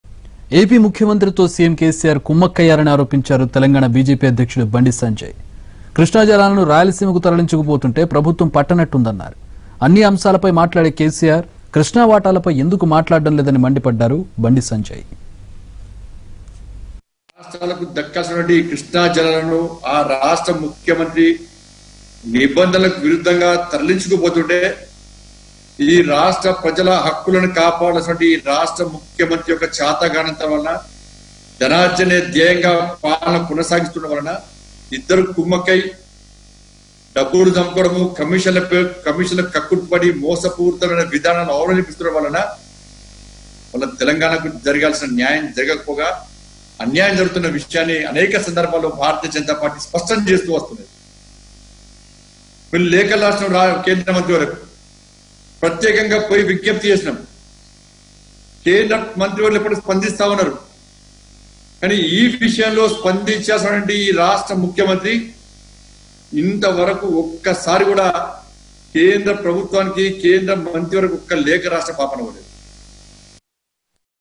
జగన్ తో కేసీఆర్ కుమ్మక్కయ్యారు: Bandi Sanjay Slams CM KCR and Jagan on Projects Issue | NTV
Bandi Sanjay Slams CM KCR and Jagan on Projects Issue